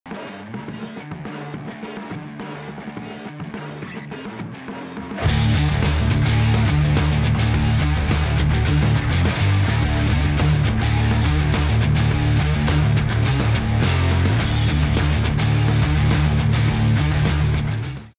Underwater Catfish Strike
Don´t blink if you want to seethestrike 👁

The pace of a catfish taking your lure is something else! Such cool recordings for our new EscapeCam, filmed by @realfishingde

The smallest & lightest underwater camera on the market is available this autumn!